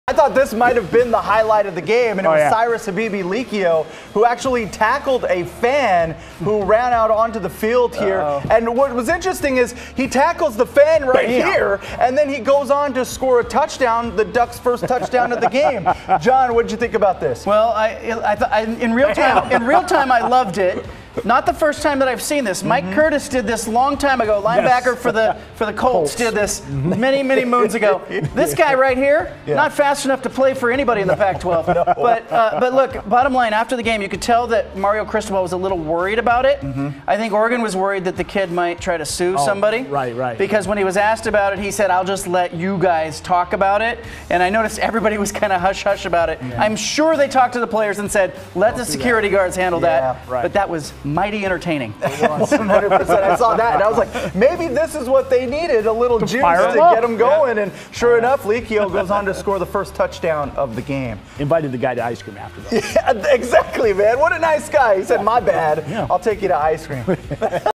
Fan runs onto field, tackled by Oregon Ducks player
Oregon Ducks running back Cyrus Habibi-Likio tackled a fan who ran on the field during Saturday's college football game against Cal. Video courtesy of Jeremy Elledge